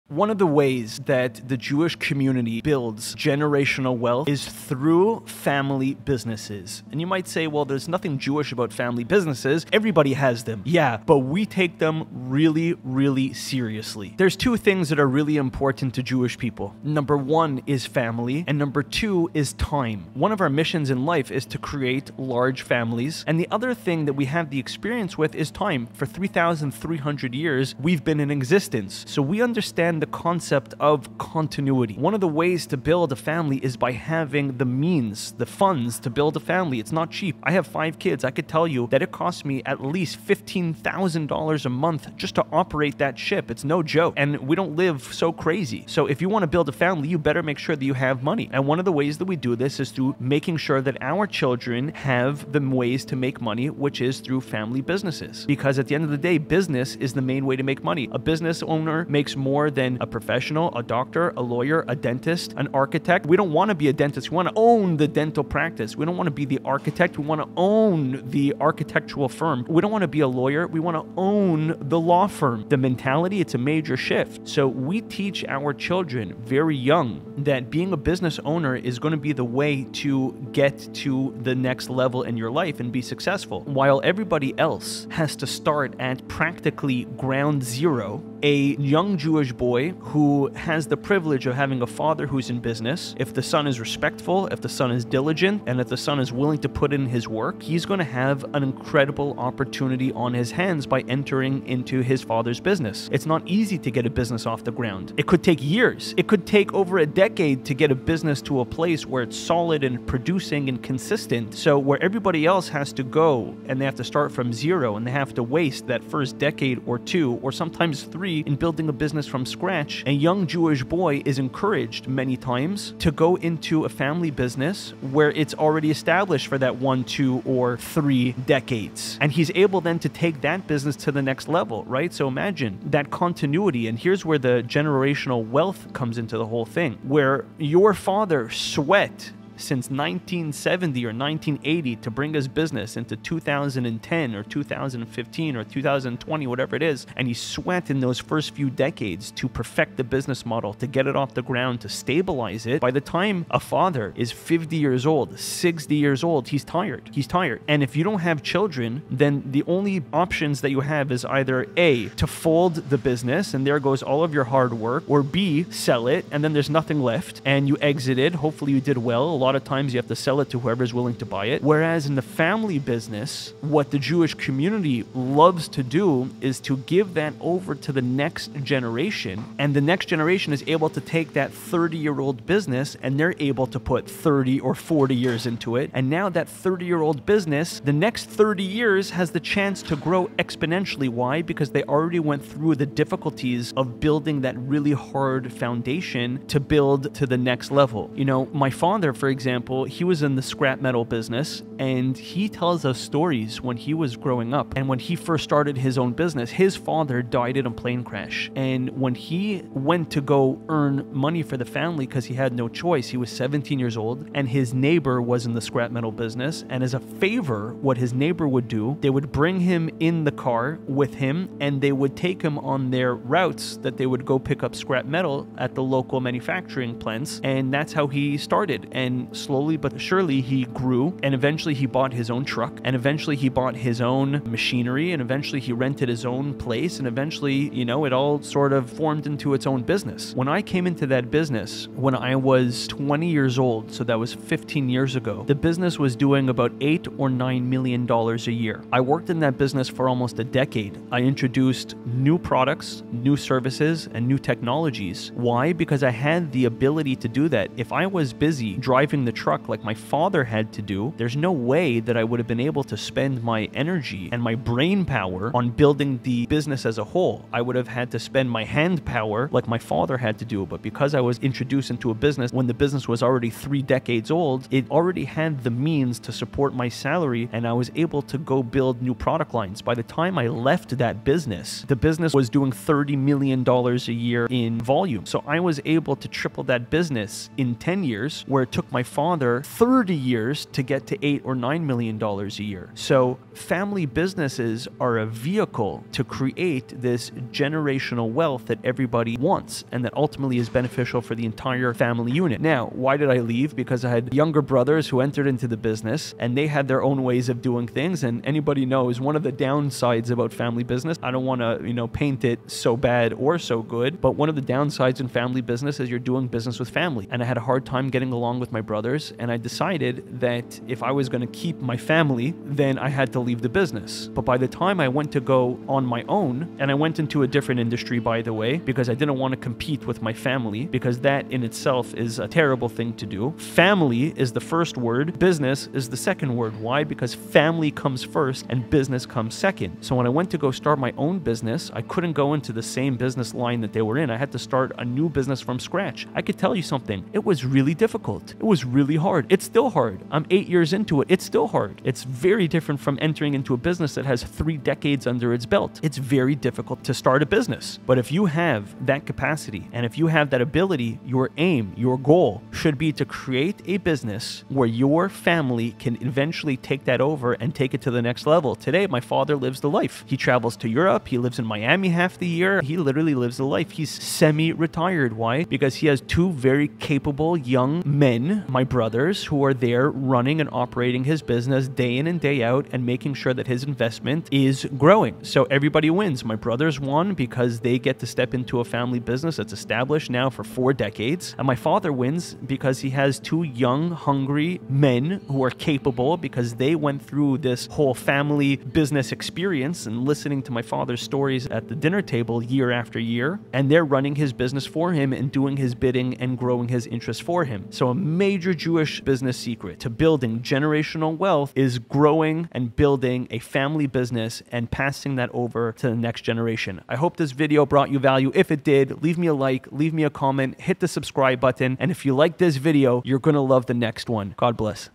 Jewish Family Business.
In this video, Jewish SECRETS to building generational wealth. Building generational wealth is a cornerstone of the Jewish community’s approach to financial success, with family businesses playing a pivotal role. As Berel explains in Jewish Secrets to Building Generational Wealth, the key lies in passing down businesses from generation to generation. This strategy allows the younger generation to build upon decades of hard work, rather than starting from scratch. While other professionals may need years to establish themselves, young Jewish entrepreneurs often enter family businesses that have been solidified for decades. This continuity accelerates growth and ensures stability, as evidenced by Berel’s own experience, where he tripled his family’s business from $9 million to $30 million in just ten years. By focusing on business ownership rather than traditional professions, Jewish families can compound wealth, ensuring financial security for future generations.

Another significant factor in Jewish generational wealth is the emphasis on family and the importance of creating opportunities for future generations. In his video, Berel shares how the Jewish community instills in its children the importance of entrepreneurship from an early age. By teaching that being a business owner provides a more secure and prosperous future, families ensure that their children are set up for success. These family businesses not only generate wealth but also provide a platform for innovation and expansion. Berel’s story highlights how entering an already-established business allows young entrepreneurs to focus on innovation and scaling, rather than building from the ground up. This long-term vision, deeply rooted in family values, serves as a powerful vehicle for wealth creation and sustenance across multiple generations.